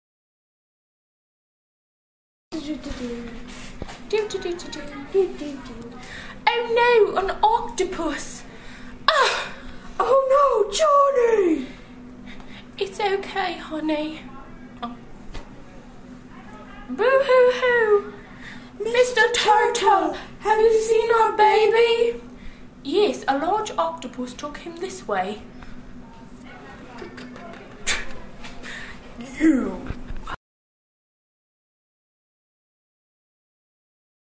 The Lost Johnny
This is a claymation was created by Sturgis Middle School 8th grade students.  Students worked together in small groups.  The ideas are completely created by the students.